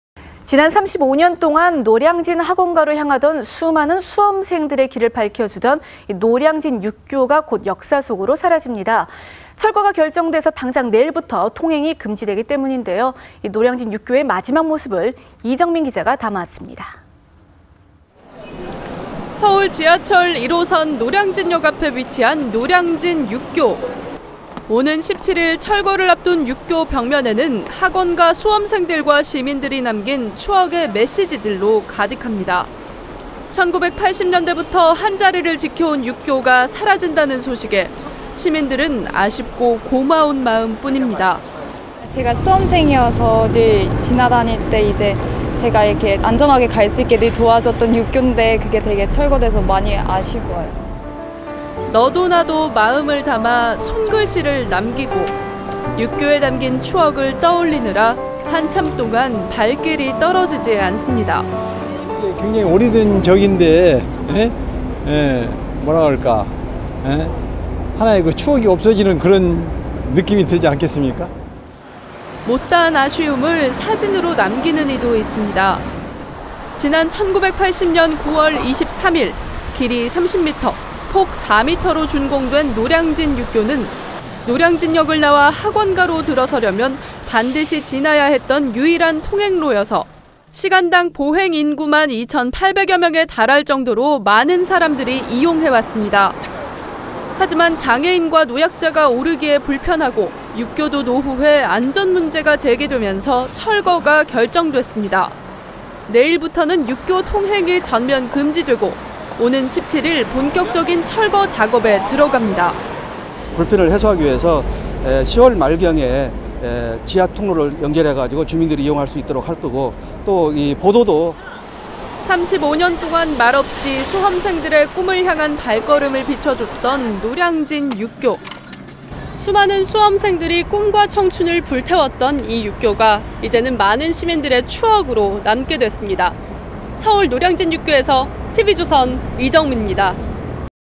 35년간 수험생들 지켜준 노량진 육교, 이제 추억속으로
지난 35년동안 노량진 학원가로 향하던 수많은 수험생들의 길을 밝혀주던 노량진 육교가 곧 역사속으로 사라집니다.